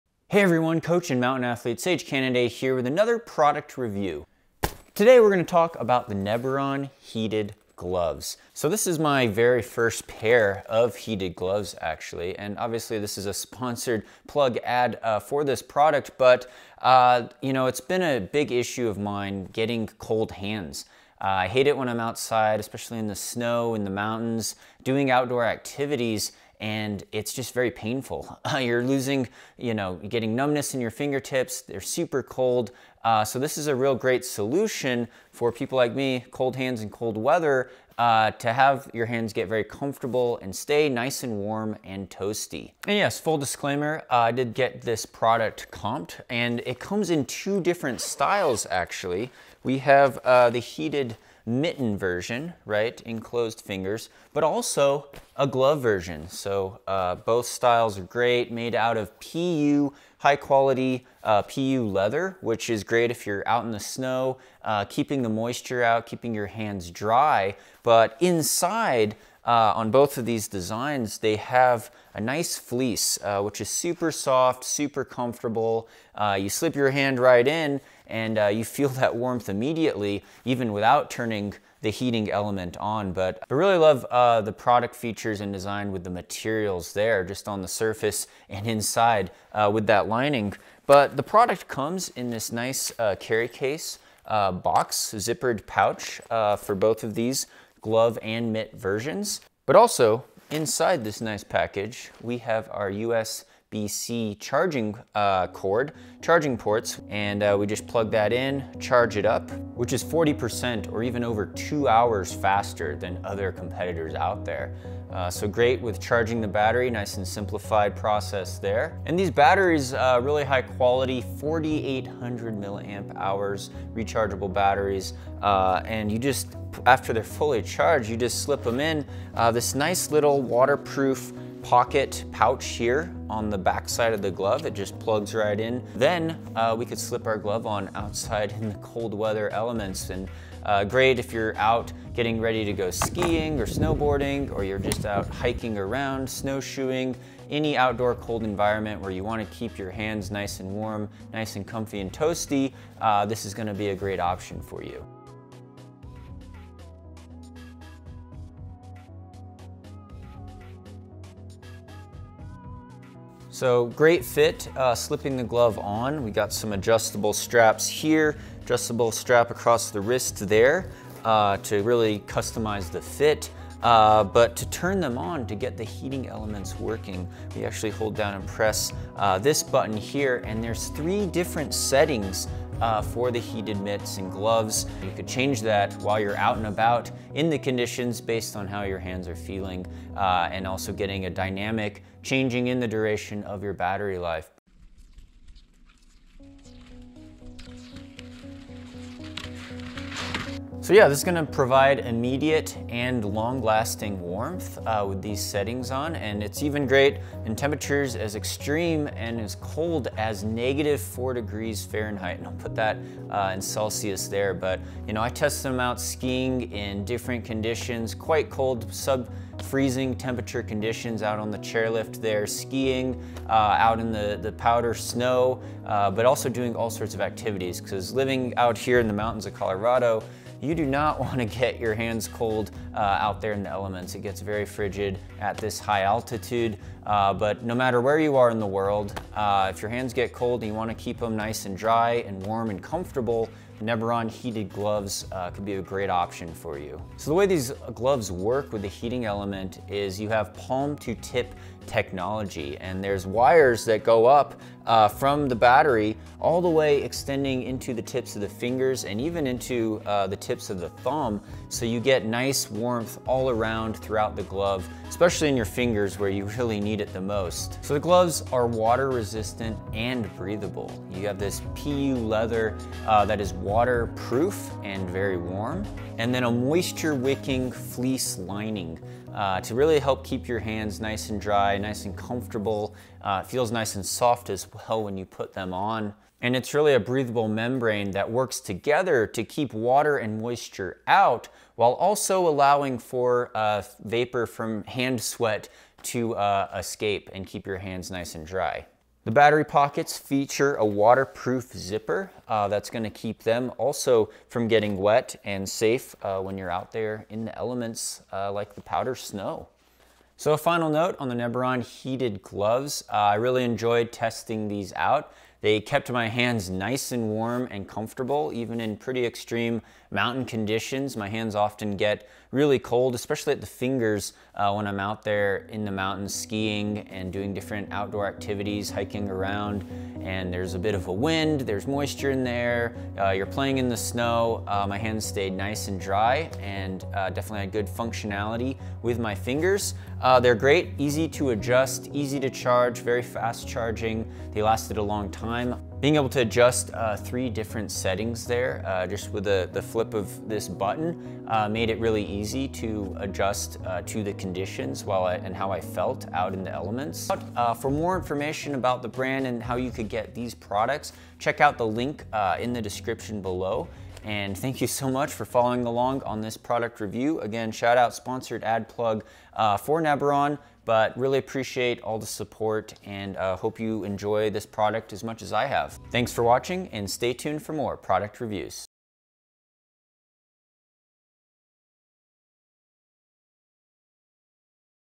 Neberon Heated Gloves Product Review! A Warm solution for the hands in cold weather, winter sports
The Neberon Heated Gloves features Palm 2Tip™ Technology and WarmKeeper features in the design. Battery powered heated gloves are a solution to keep your hands and fingers warm in very cold winter weather conditions and doing all outdoor activities!